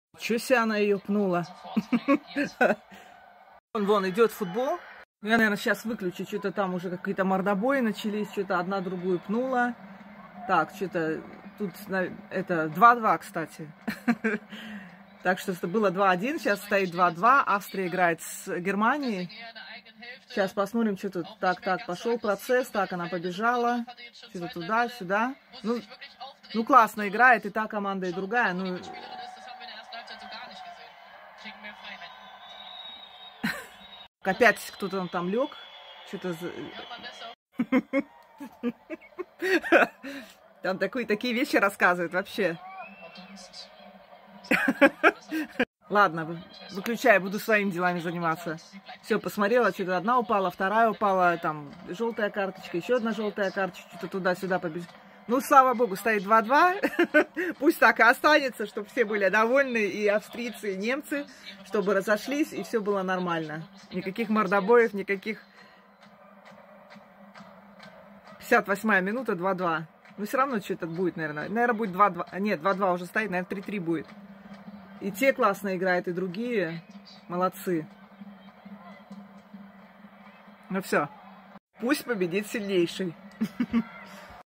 Germany Vs. Austria Women's Football Match On 5th April 2024
Germany Vs. Austria Women's Football Match On 5th April 2024. Video Maria Marachowska 2024

Maria Marachowska works as a freelance artist both in the fields of art  @MARACHOWSKAART                     (since 1996) and music  @siberianbluesberlin    (since 2004) in Berlin.